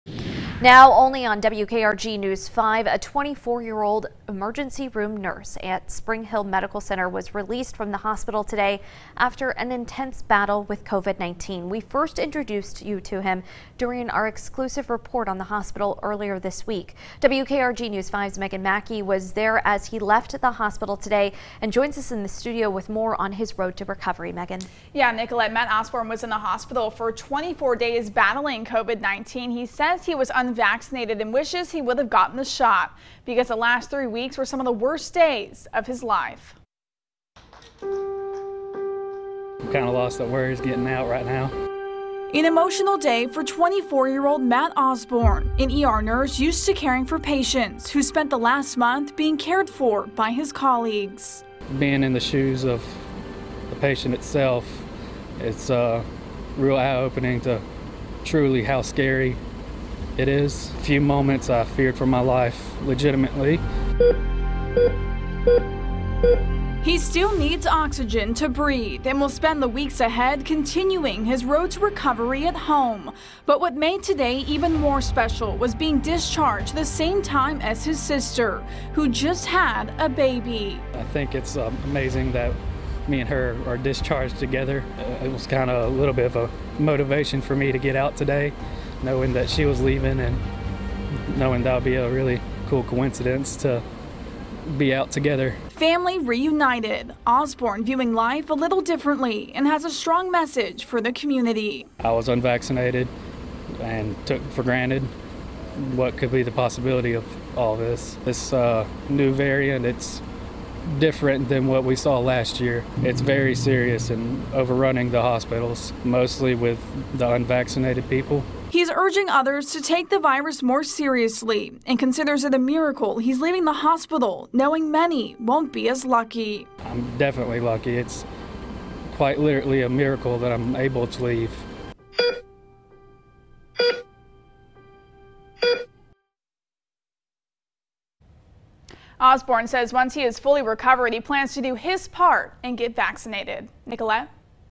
Unvaccinated ER nurse discharged from hospital after battling COVID-19, calls for others to get the
Unvaccinated ER nurse discharged from hospital after battling COVID-19, calls for others to get the shot